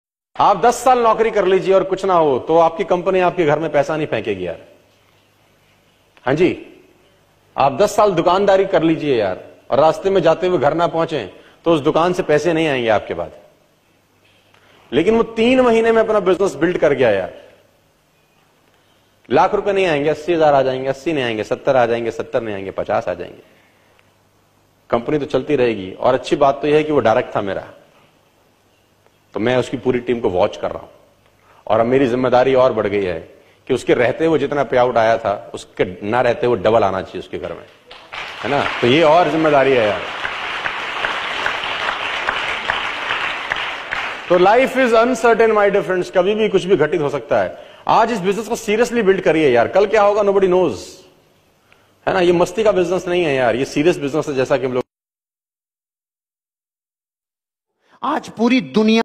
नेट्वोर्किङ ब्यावसाय गर्नुहोस|सदै पैसा आइरहन्छ | तर नोकरी जती बर्ष गरेपनि नोकरी छोडेपछि पैसा आउदैन।
नेट्वोर्ककिङ गर्नुहोस जिन्दगी बदल्नु छ भने